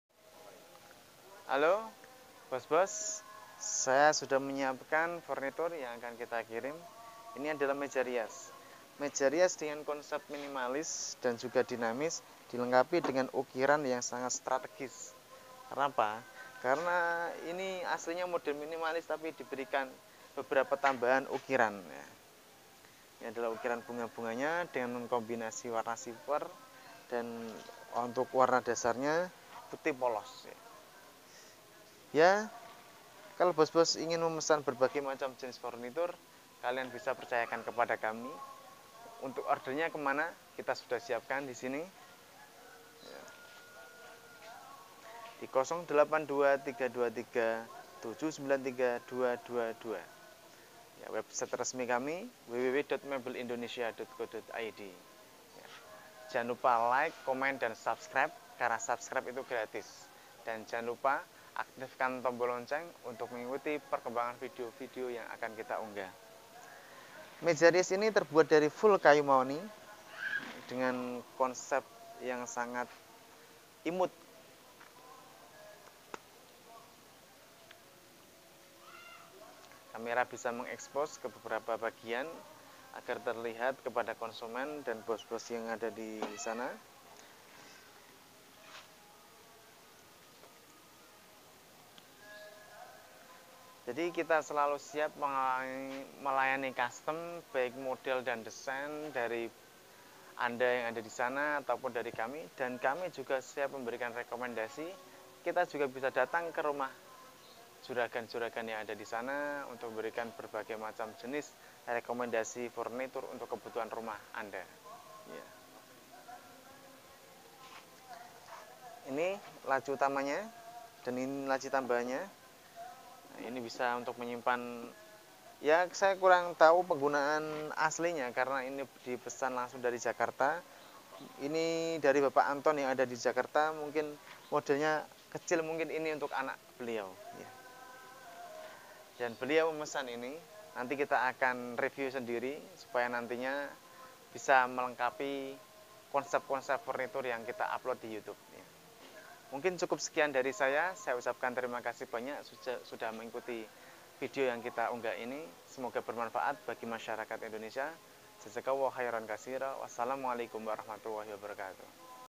Merja Rias Anak cat Duco Putih
SHOWROOM & WORKSHOP:
Modelnya yang imut sangat cocok untuk anak-anak, untuk ukuran dan desainnya bisa disesuaikan sesuai permintaan, warnanya juga bisa dikombinasikan sesuai keinginan Anda.